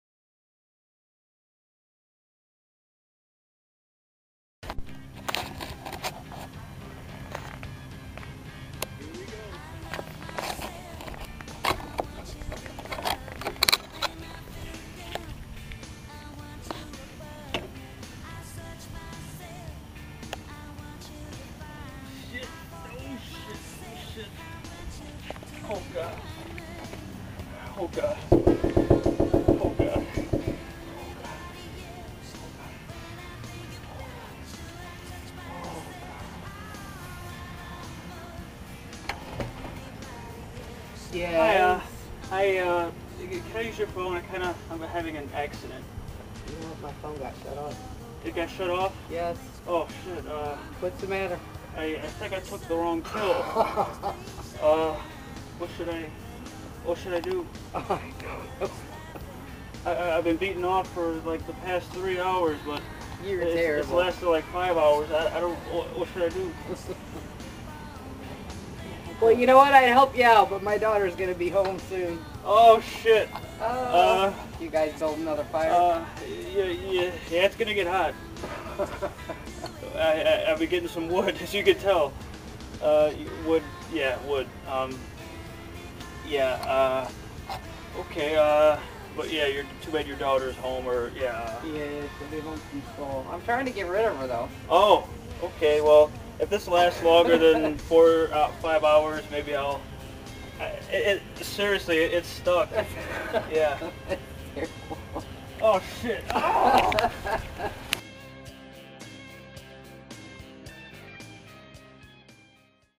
jackass tribute Stunt #2 Neighborly Help
Outsider goes to neighbor's house for help with erection problem.